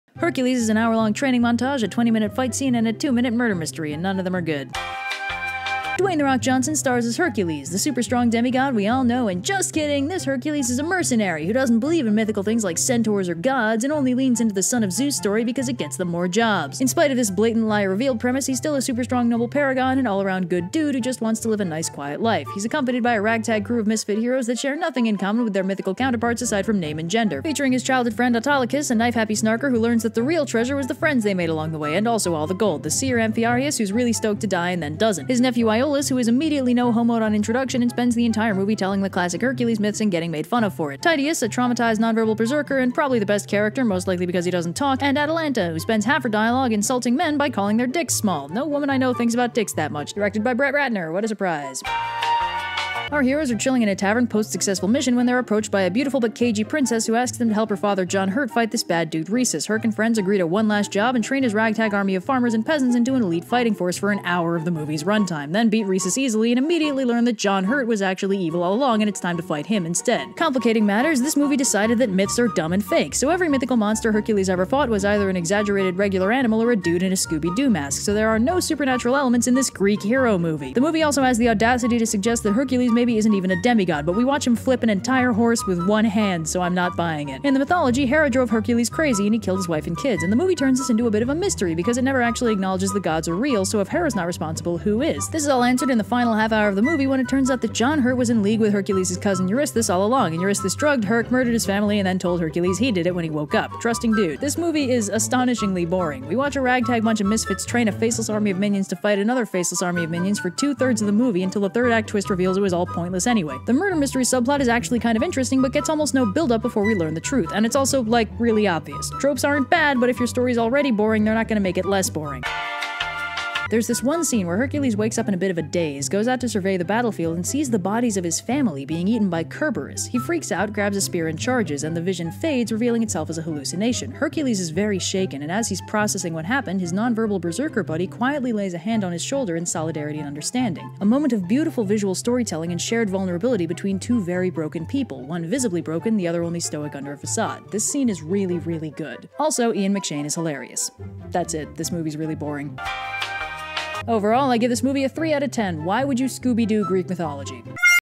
Summer Stupidity: HERCULES 2014 (Media Review!)
"I've seen The Rock act before and he's really fun and charismatic! Surely this movie starring him will be a Good Time!" I thought, foolishly.